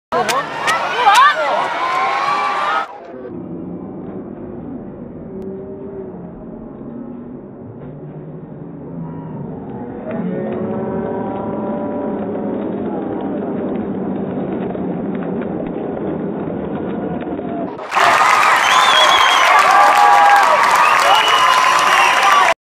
England vs Montenegro Women's World Cup Qualifier.
Toni Duggan scoring her Hat-trick goal at the Amex.